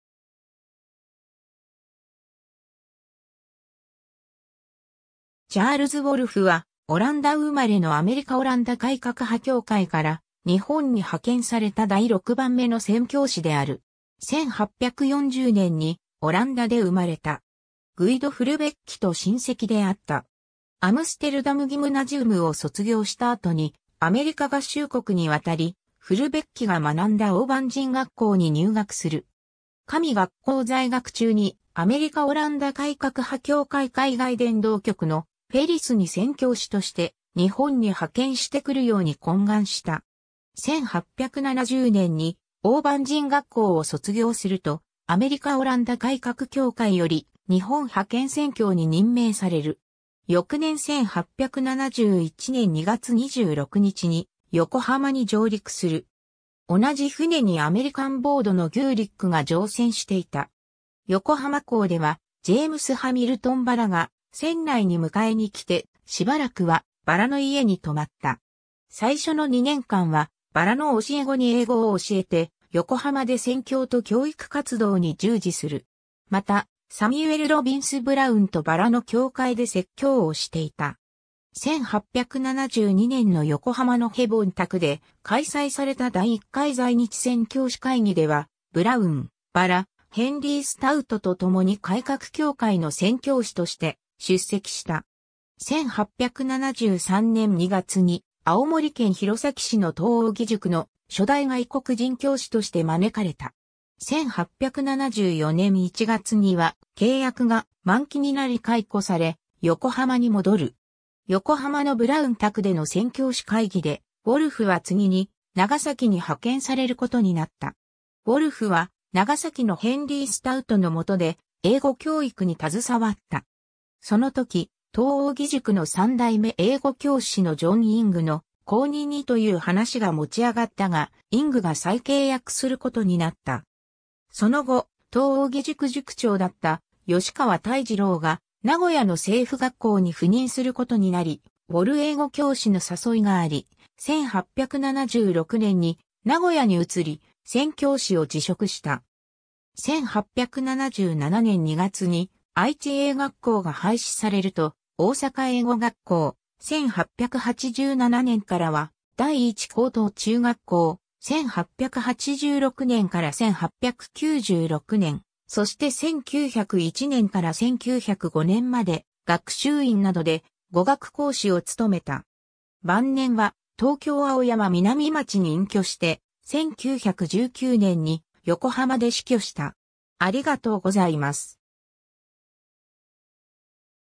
チャールズ・ウォルフ (宣教師)
チャールズ・ウォルフ（Charles Wolff、1840年8月20日 - 1919年8月26日）は、オランダ生まれのアメリカ・オランダ改革派教会から日本に派遣された第6番目の宣教師である。
 1840年にオランダで生まれた。
グイド・フルベッキと親戚であった。
アムステルダム・ギムナジウムを卒業した後に、アメリカ合衆国に渡り、フルベッキが学んだオーバン神学校に入学する。
神学校在学中にアメリカ・オランダ改革派教会海外伝道局のフェリスに宣教師として日本に派遣してくるように懇願した。
 1870年にオーバン神学校を卒業すると、アメリカ・オランダ改革教会より日本派遣宣教に任命される。
翌年1871年2月26日に横浜に上陸する。
同じ船にアメリカン・ボードのギューリックが乗船していた。
横浜港ではジェームス・ハミルトン・バラが船内に迎えに来て、しばらくはバラの家に留まった。
最初の2年間はバラの教え子に英語を教えて、横浜で宣教と教育活動に従事する。
また、サミュエル・ロビンス・ブラウンとバラの教会で説教をしていた。
 1872年の横浜のヘボン宅で開催された第一回在日宣教師会議ではブラウン、バラ、ヘンリー・スタウトと共に改革教会の宣教師として出席した。
 1873年2月に青森県弘前市の東奥義塾の初代外国人教師として招かれた。
1874年1月には契約が満期になり解雇され、横浜に戻る。
横浜のブラウン宅での宣教師会議で、ウォルフは次に長崎に派遣されることになった。
ウォルフは長崎のヘンリー・スタウトの元で英語教育に携わった。
その時、東奥義塾の3代目英語教師のジョン・イングの後任にという話が持ち上がったが、イングが再契約することになった。
 その後、東奥義塾塾長だった吉川泰次郎が名古屋の政府学校(愛知英語学校)に赴任することになり、ウォル英語教師の誘いがあり、1876年に名古屋に移り、宣教師を辞職した。
1877年2月に愛知英学校が廃止されると、大阪英語学校、1887年からは第一高等中学校、1886年から1896年、そして1901年から1905年まで学習院などで語学講師を務めた。
 晩年は東京青山南町に隠居して、1919年に横浜で死去した。